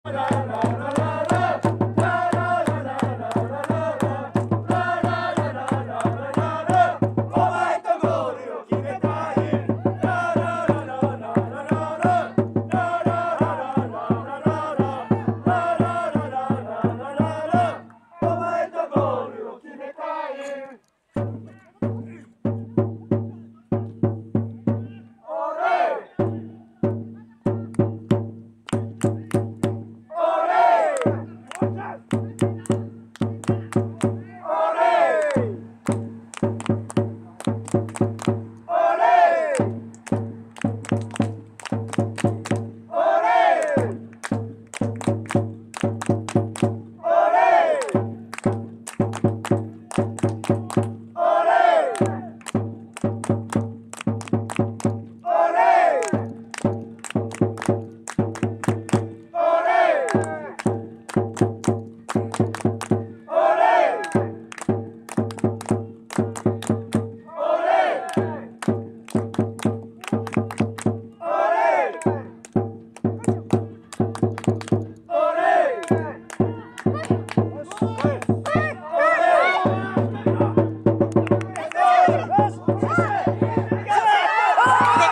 2011ＪＦＬ前期第2節 横河武蔵vsブラウブリッツ秋田 松田正俊の1点目
2011年12月3日　西が丘で行われた前期第2節　横河武蔵戦　67分の松田のゴール